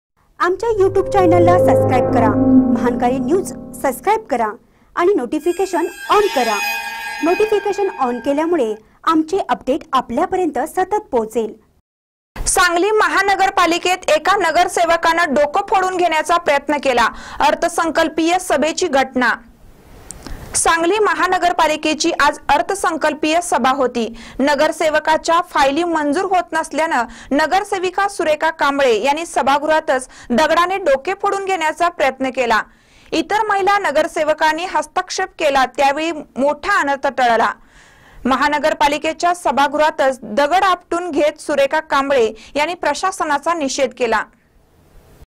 सांगली महानगरपालिकेत एका नगसेविकेनं डोकं फोडून घेण्याचा प्रयत्न
सांगली महानगरपालिकेत एका नगसेविकेनं डोकं फोडून घेण्याचा प्रयत्न केला अर्थसंकल्पीय सभेची घटना